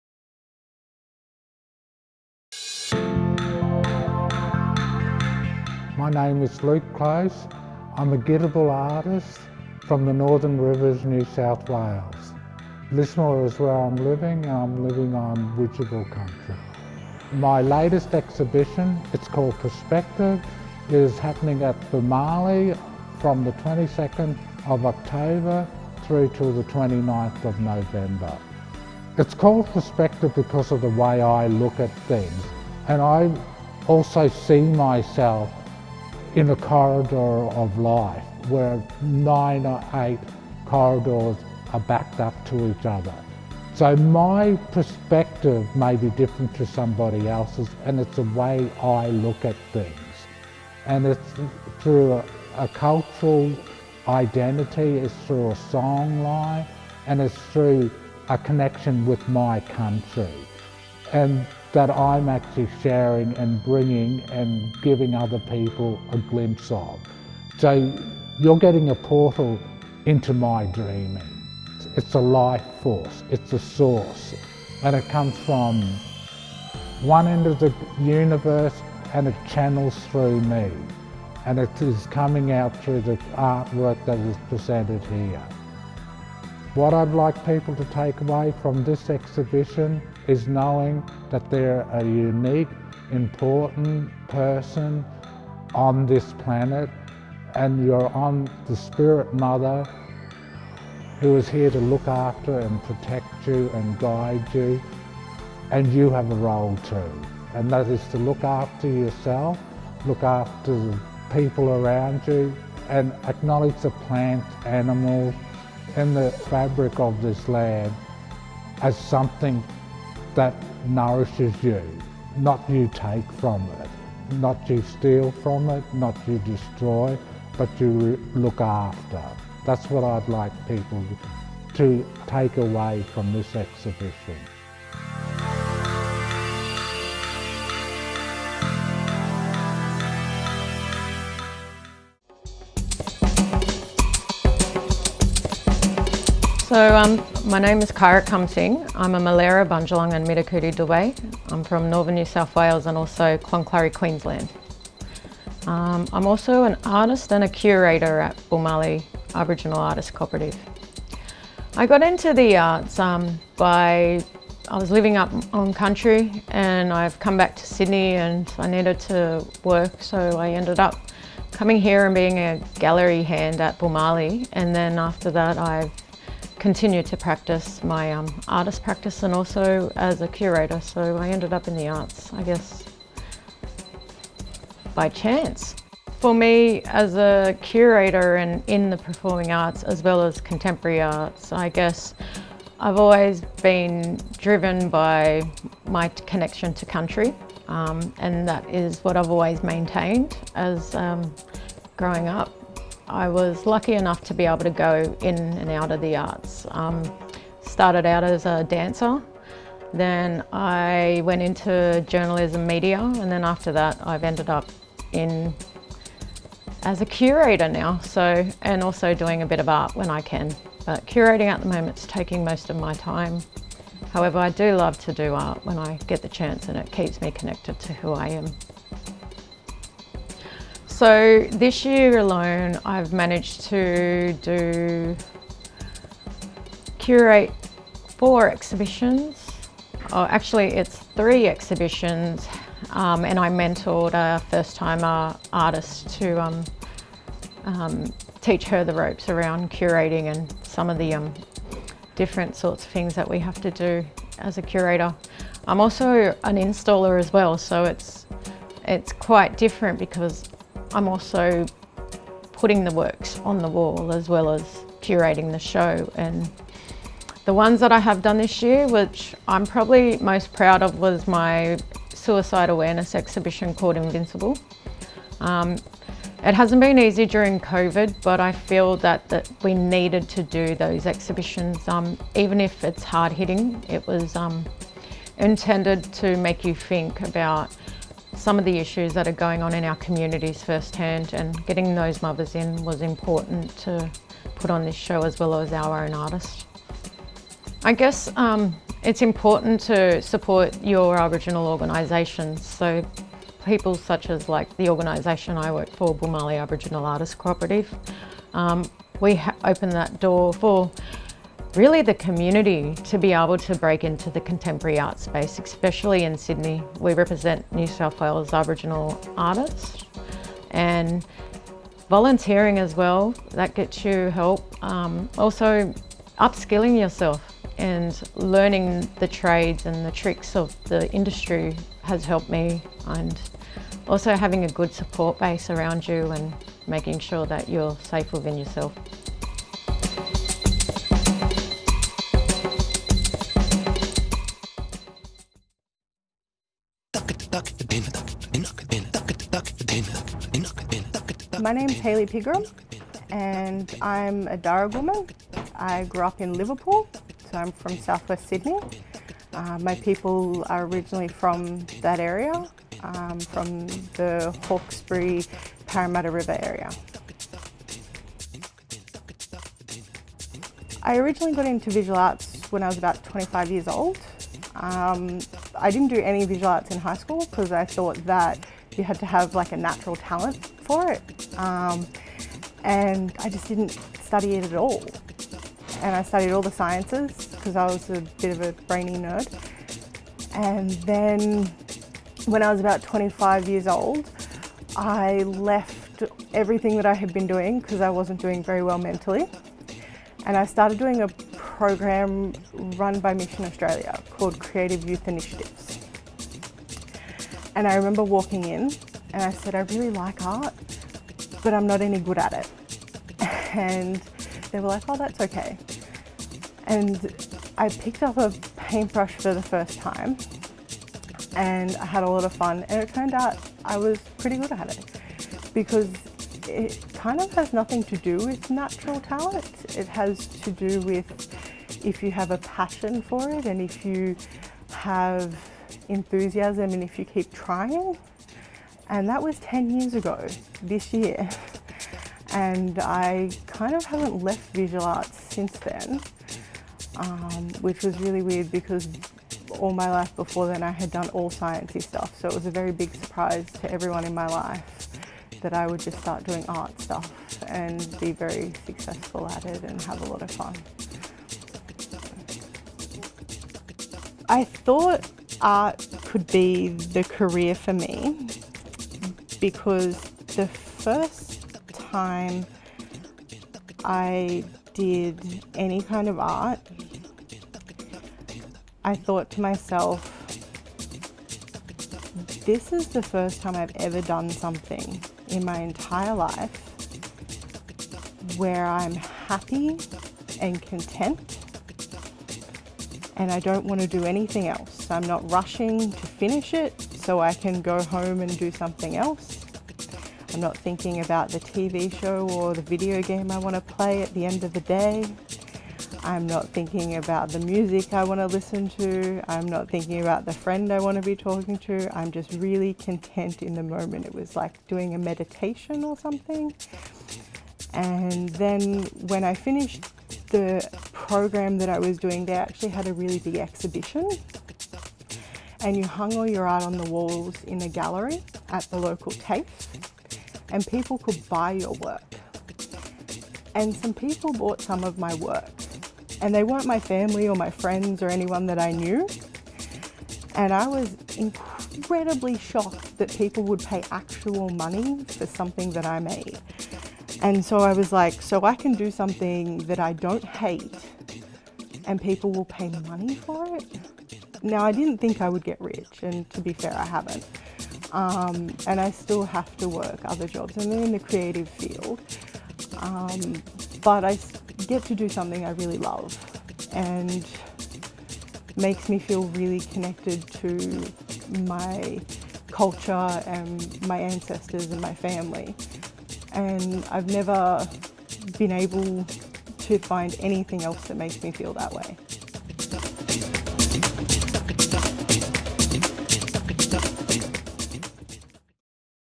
Boomalli Aboriginal Artists Co-operative - Parramatta Lanes
See and hear from Aboriginal artists from the Boomalli Aboriginal Artists Co-operative and take a fascinating look into their works and exhibitions. Includes Luke Close's Solo Exhibition: Perspective, and artists Hayley Pigram Dossier and Kyra Kum-Sing. Presented as part of Parramatta Lanes festival